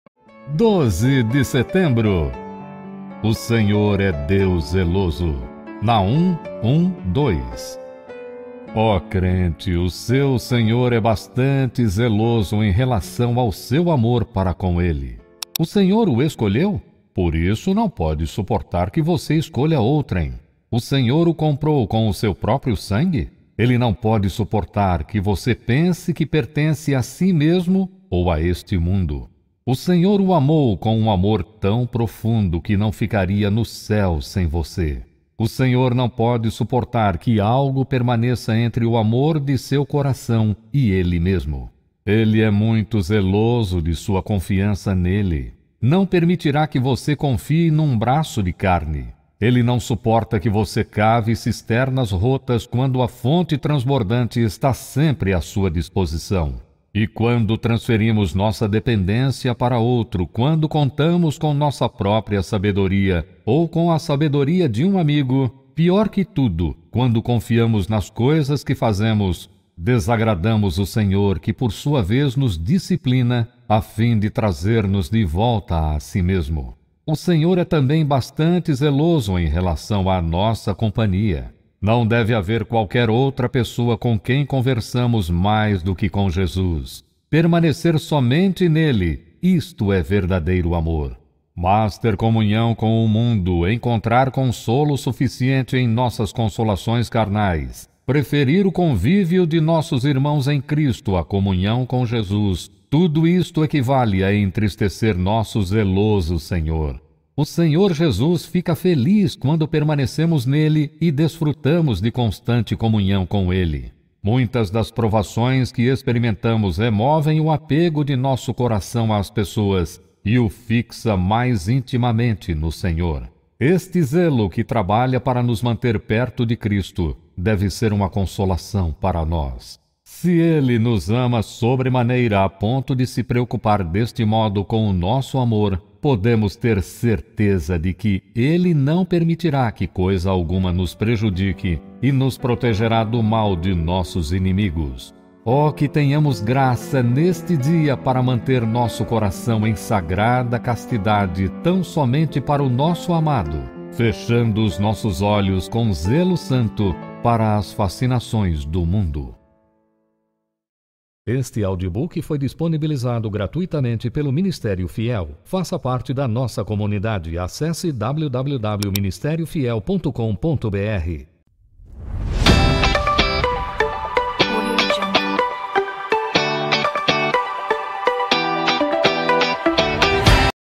12 de Setembro Devocional Diário Charles Spurgeon
Fonte: @Ministeriofiel

12 de Setembro

 O SENHOR é Deus zeloso.

Naum 1.2